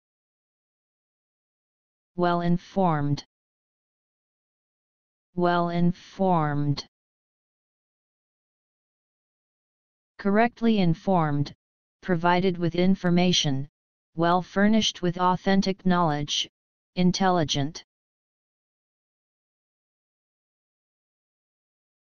Well informed Meaning
Correctly informed; provided with information; well furnished with authentic knowledge; intelligent.

In this video, you will learn ""Well-informed"" Meaning and pronunciation in English.

Wordy Affair